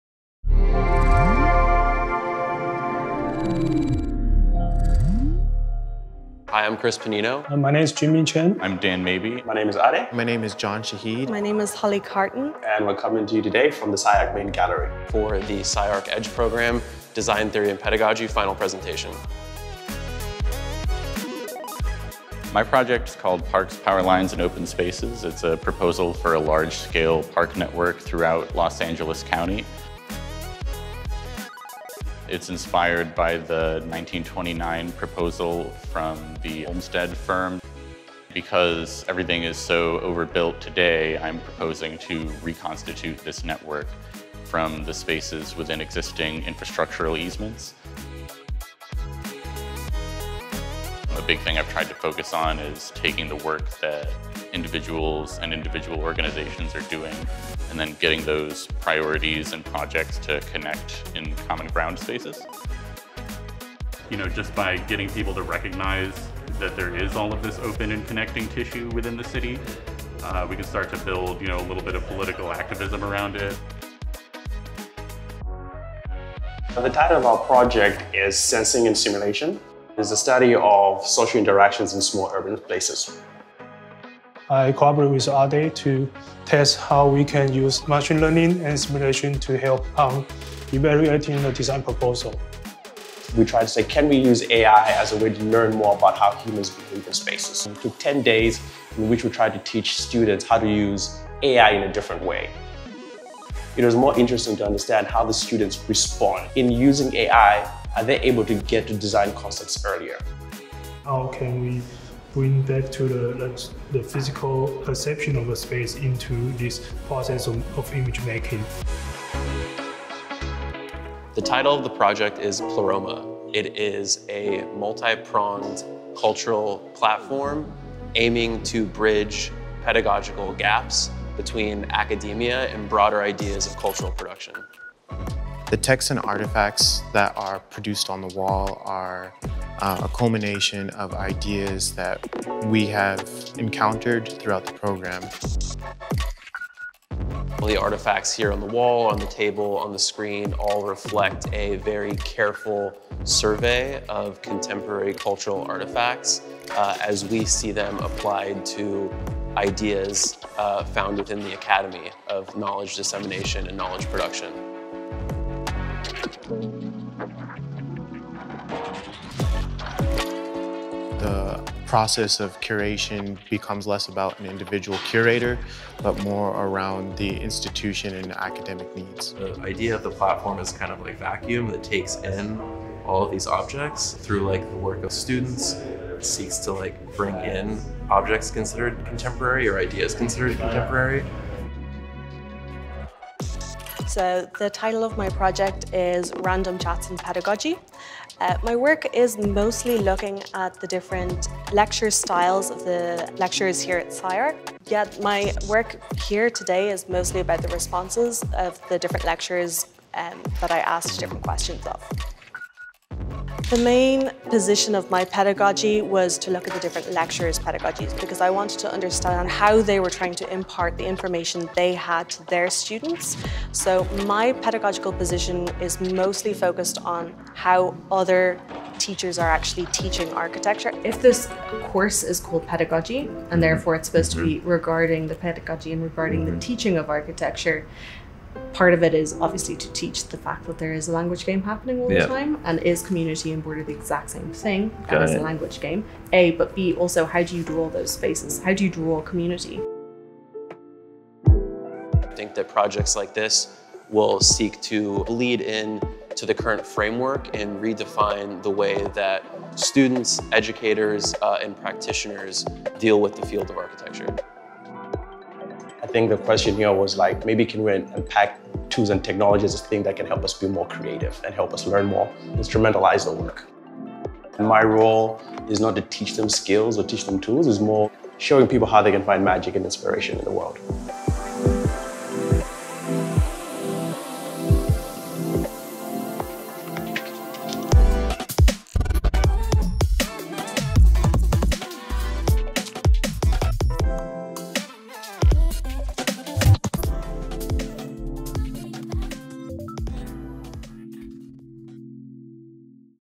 Imparting Knowledge: SCI-Arc EDGE MS Design Theory & Pedagogy
SCI-Arc EDGE Design Theory & Pedagogy 2023 graduates speak about the projects, research, and pedagogical models they developed in the context of the one-year MS Design Theory & Pedagogy program at SCI-Arc. The Master of Science in Design Theory and Pedagogy is a one-year, three-semester program that addresses the growing ambiguity between practice and academia and prepares students for a new kind of hybrid career that has emerged in architecture: the architect-theorist-educator. Projects included in the 2023 EDGE Design Theory & Pedagogy year-end exhibition held in the SCI-Arc Gallery address the role of AI, machine learning, and simulation in the design process; contemporary forms of architectural pedagogy; creating urban networks of public community spaces; and implementing a custom platform for bridging academia with broader notions of cultural production.

For more information about SCI-Arc's EDGE MS Design Theory & Pedagogy program